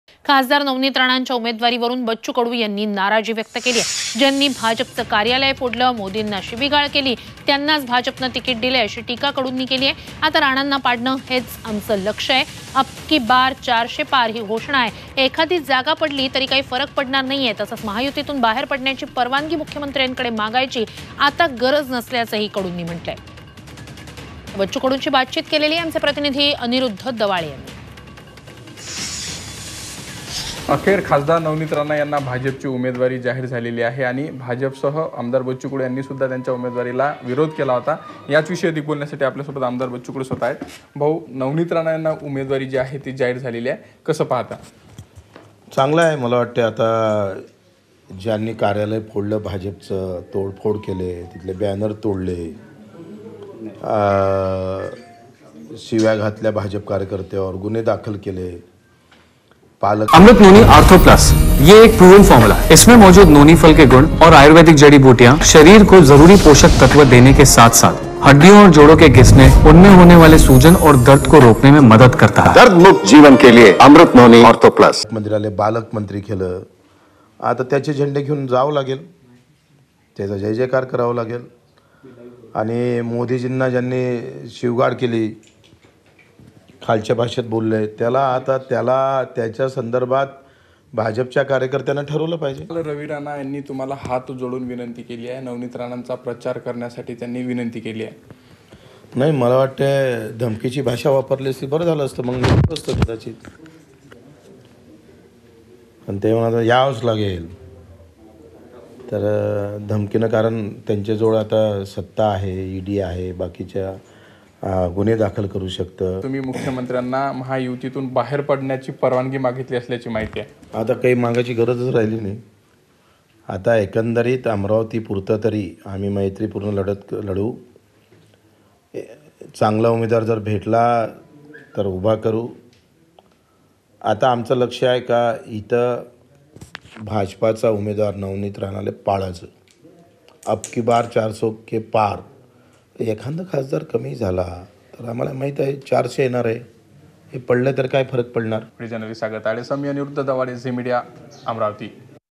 Bacchu Kadu On Navneet Rana | 'नवनीत राणा यांना पाडणं हेच लक्ष'; बच्चू कडू यांचा थेट आक्रमक
MLA | Bacchu Kadu - Uncut On Contest Independent For Amravati Lok Sabha Constitutency

खासदार नवनीत राणांच्या उमेदवारीवरून बच्चू कडूंनी नाराजी व्यक्त केलीय...
ज्यांनी भाजपचे कार्यालय फोडलं, मोदींना शिवीगाळ केली त्यांनाच भाजपने तिकीट दिलं, अशी टीका कडूंनी केलीय...आता राणांना पाडणं हेच आमचं लक्ष आहे...अबकी बार चारशे पार ही घोषणा आहे...एखादी जागा पडली तरी काही फरक पडणार नाही...तसंच महायुतीतून बाहेर पडण्याची परवानगी मुख्यमंत्र्यांकडे मागायची आता गरज नसल्याचं कडूंनी म्हटलंय...

Bachu Kadu has expressed his displeasure over the candidature of MP Navneet Rana...
Bitterly criticized that BJP gave ticket only to those who broke BJP office, abused Modi...now our aim is to topple Rana...abki bar charshey par is the slogan...even if one seat falls, it will not matter.. Also, Kadu has said that there is no need to seek permission from the Chief Minister to withdraw from the Grand Alliance...